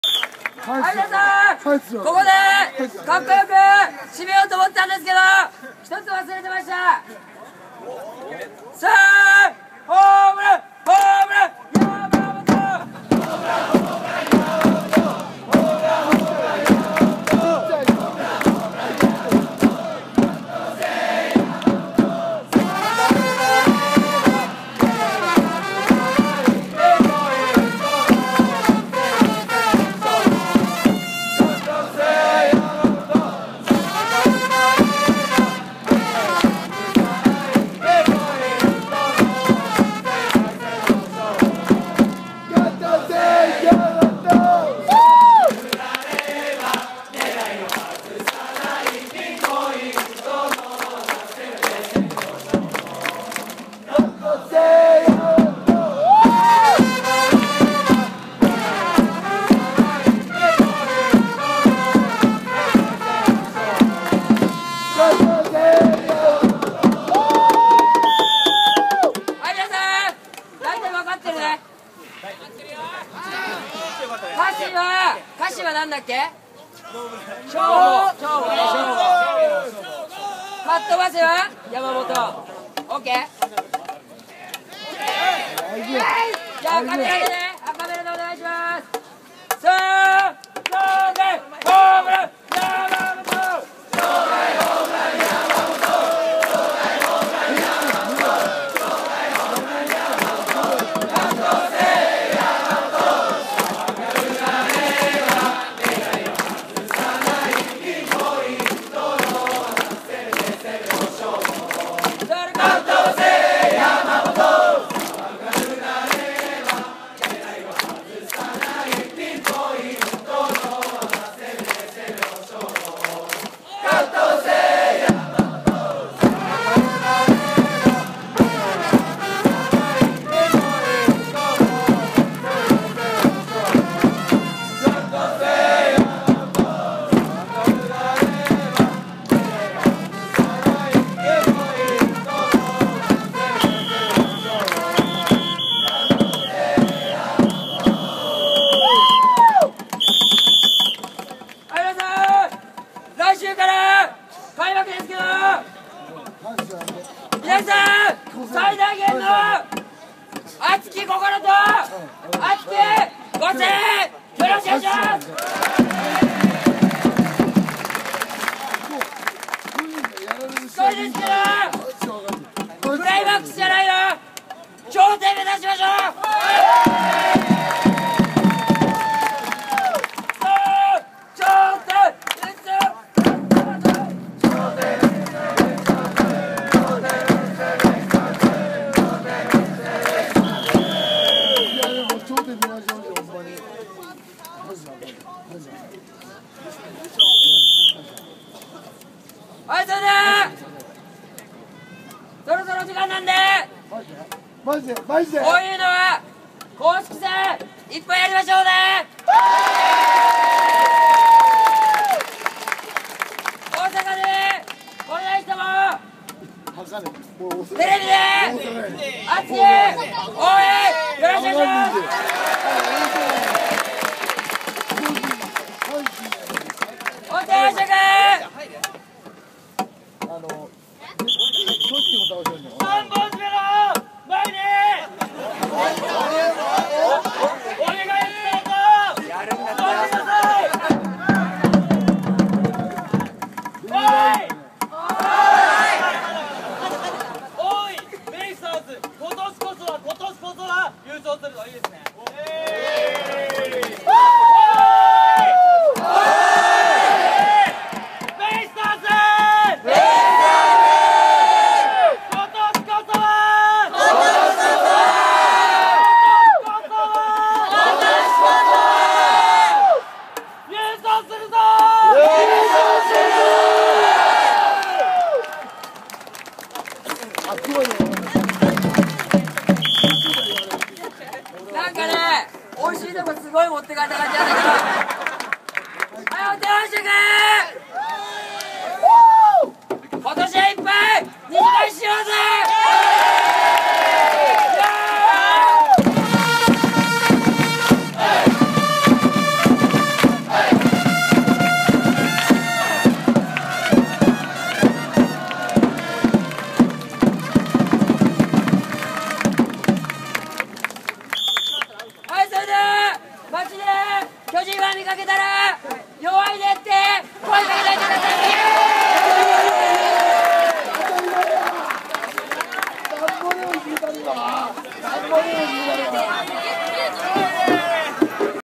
横浜ベイスターズ 二次会 2012年3月24日 (2/2) オープン戦
横浜DeNAベイスターズ対読売ジャイアンツ 横浜スタジアム

日本人先発左腕のテーマ

横浜ベイスターズ 二次会 2012年3月24日 (1/2) オープン戦